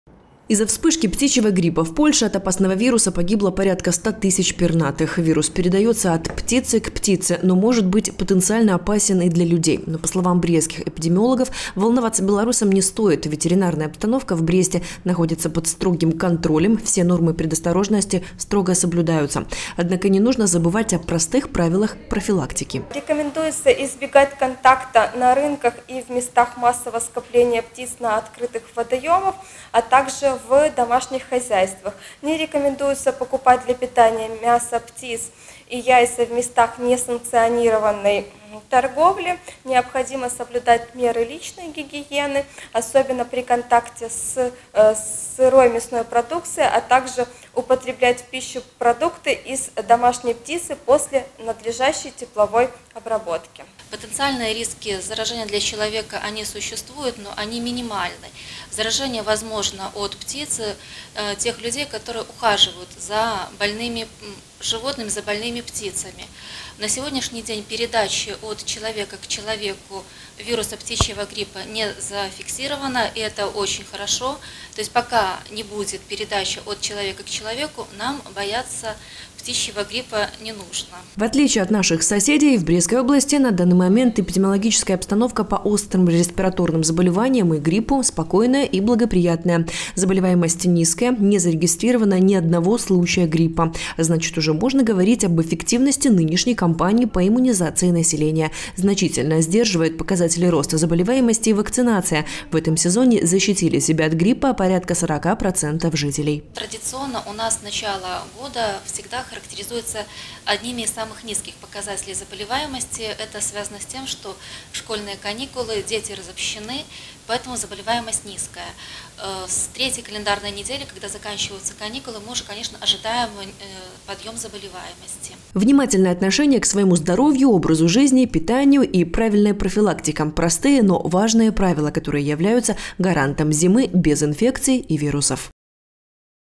2020-01-08 г. Брест. Птичий грипп: меры профилактики. Новости на Буг-ТВ.
Вспышка птичьего гриппа произошла в Польше. Вирус крайне заразен и моментально передается от птицы к птице. Кроме этого, медики считают, что заразная болезнь может быть потенциально опасна и для человека. О мерах профилактики и сложившейся эпидемиологической ситуации в Бресте и области, расскажем в следующем материале выпуска.